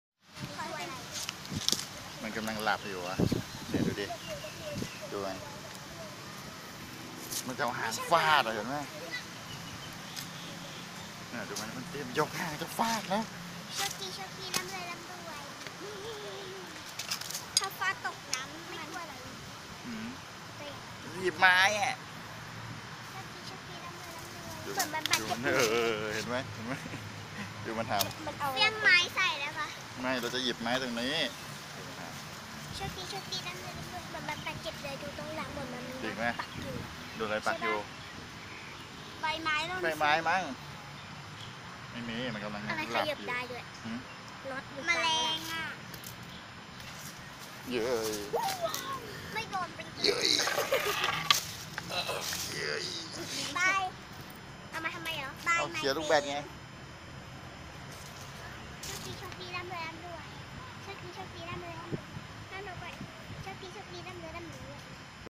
A monitor lizard kick
See how a monitor lizard did when it feels annoying by human.